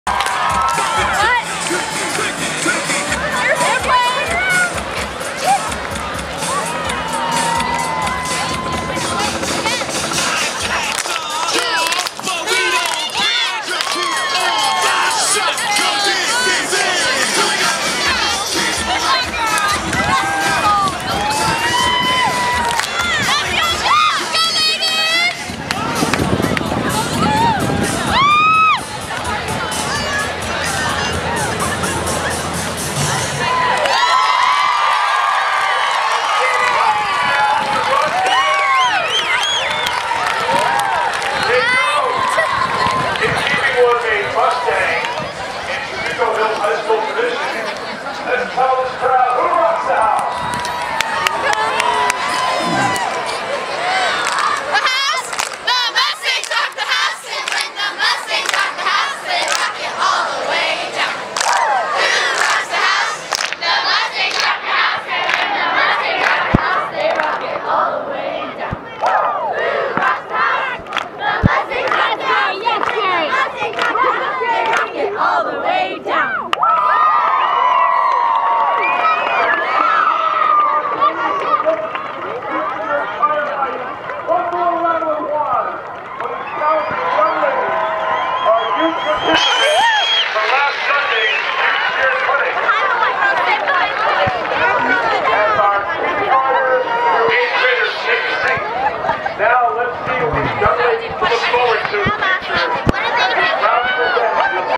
THHS Cheer training camp halftime routine 2013
See the future THHS Cheer squad hopefuls train with their cheer squad mentors in this halftime routine 11/01/2013